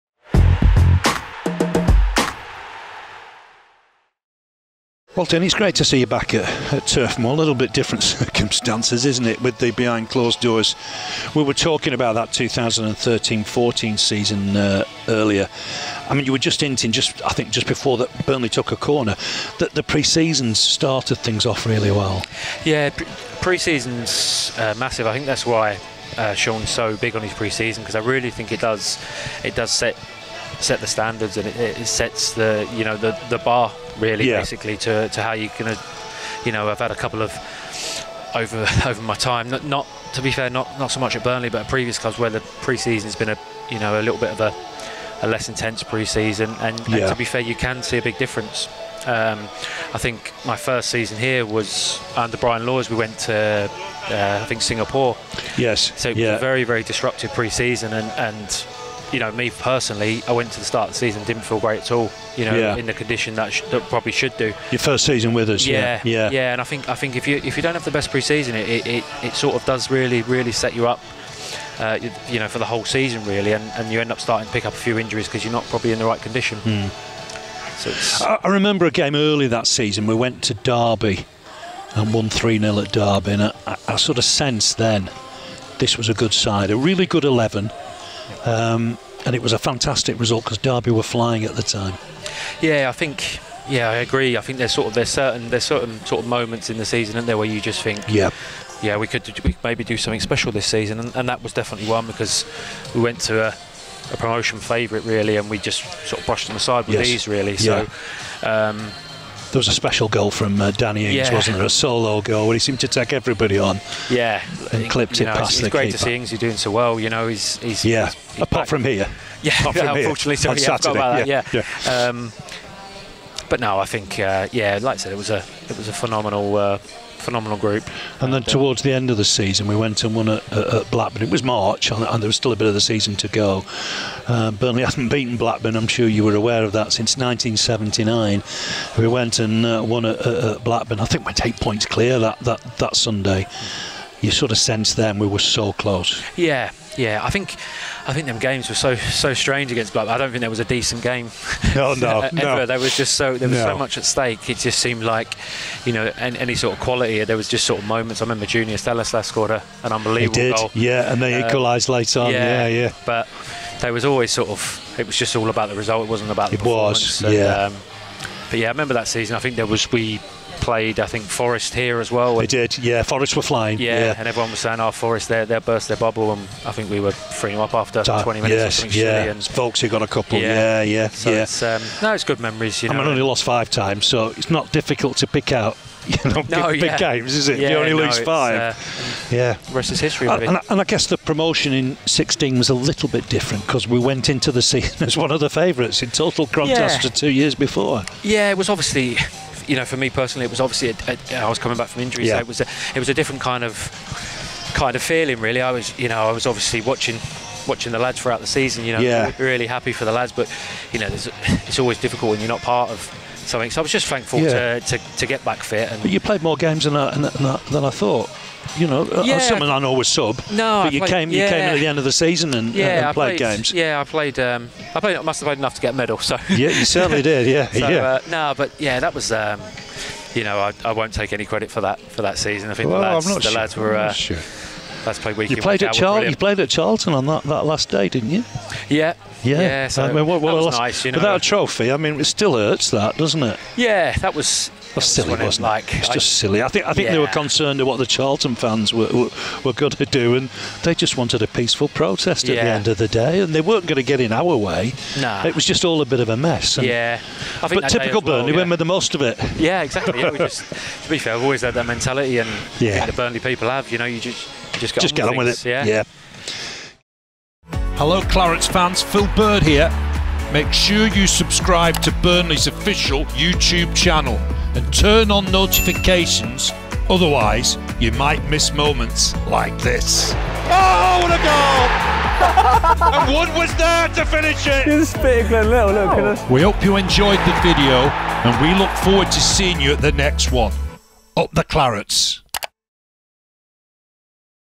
Dean Marney | HALF-TIME | Burnley v Man City
Former Clarets midfielder Dean Marney catches up with Phil Bird during half-time.